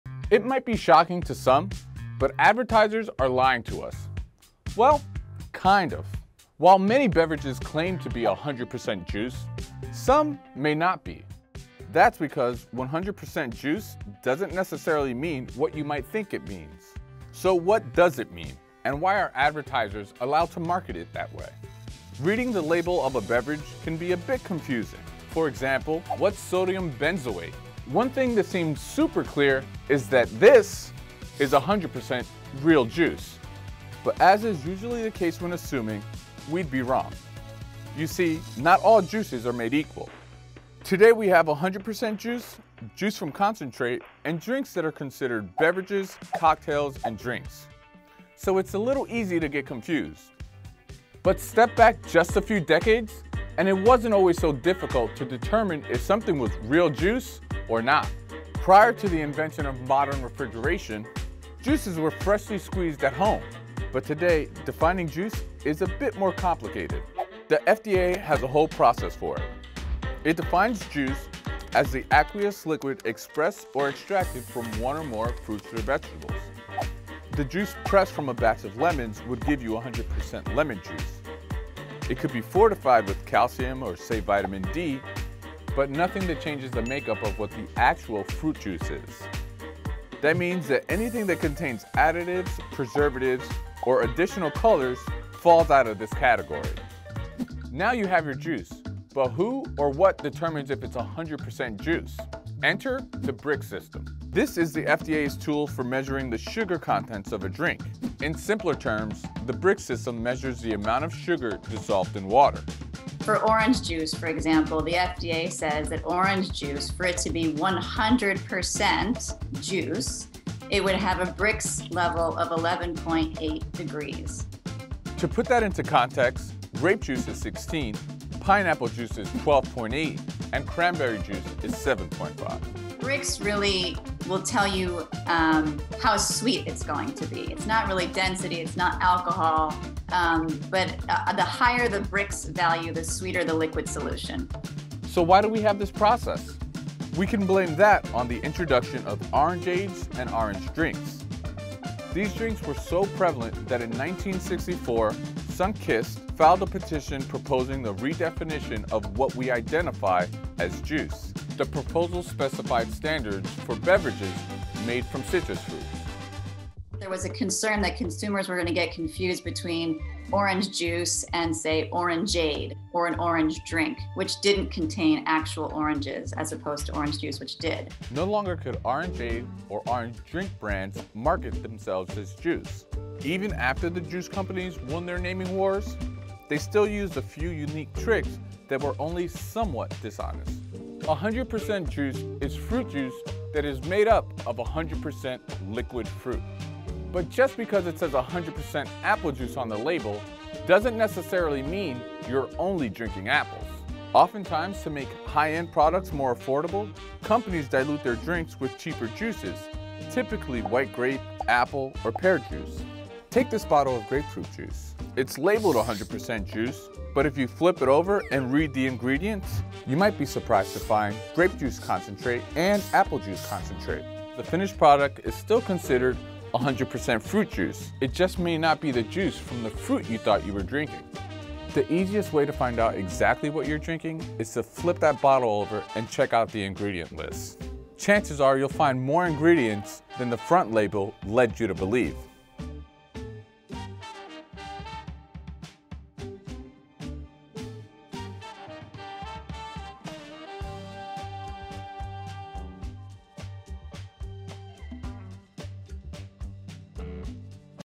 Why 100% Real Juice Is A Lie - Cheddar Explains
We’ve all gone to the grocery store and picked up a drink labeled 100% juice. Most would assume that juice is made entirely from the fruit on the front label - and most would be wrong. Here’s why.

Connect with Cheddar!